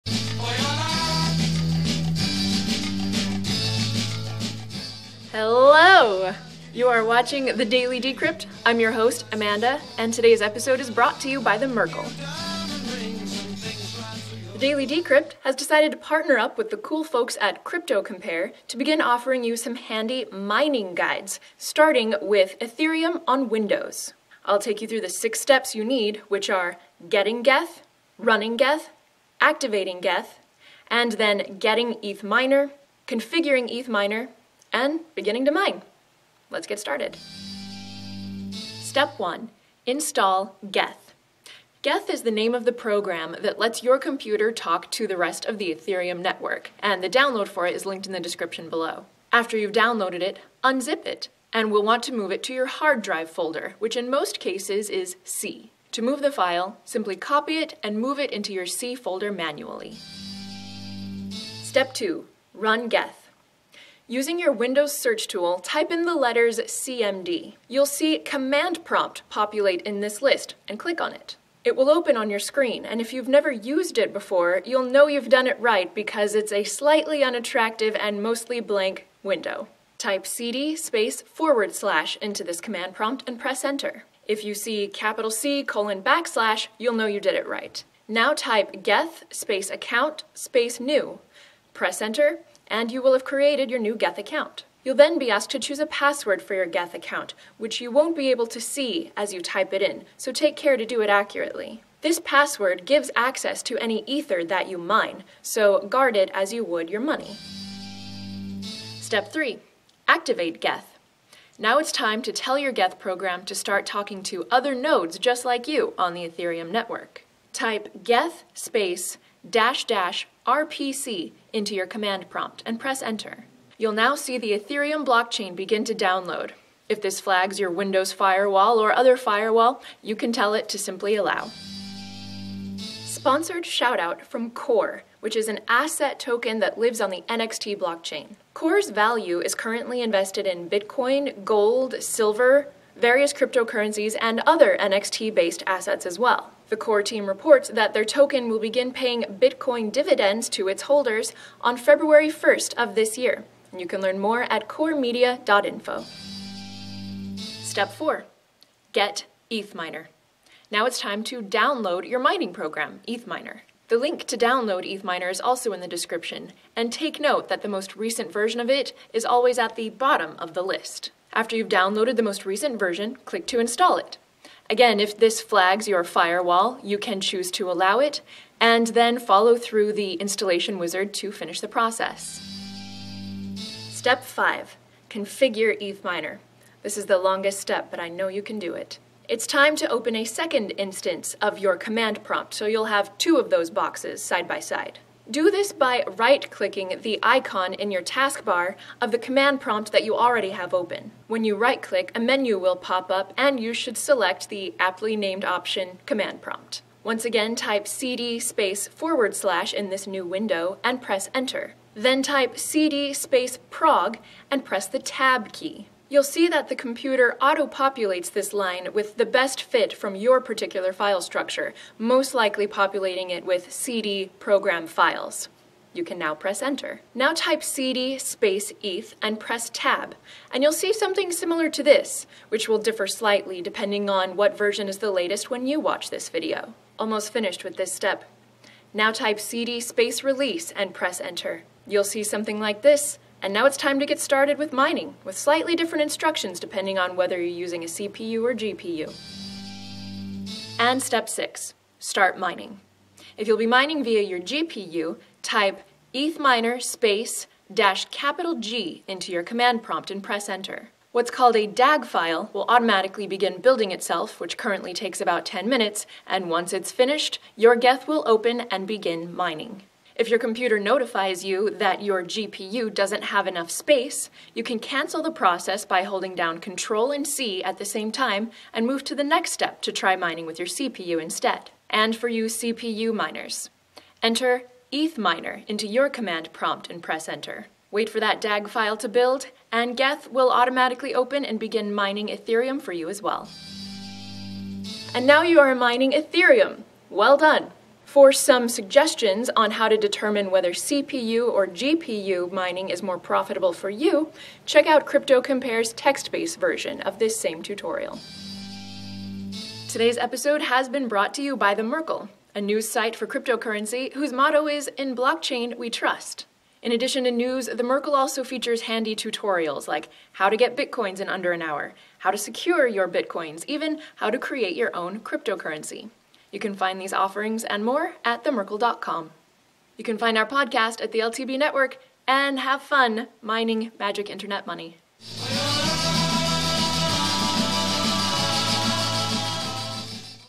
How to Mine Ethereum on a Windows PC -- 6 Steps
How to mine Ethereum? It's a little tech-y, but if you can follow simple instructions, you can make money mining Ethereum on a Windows PC. This is the first cryptocurrency mining guide from a collaboration of The Daily Decrypt with CryptoCompare -- watch for more guides to come!

UPDATE: Ethereum has hardforked and this tutorial is no longer current. to download the latest mining software.

Tip us!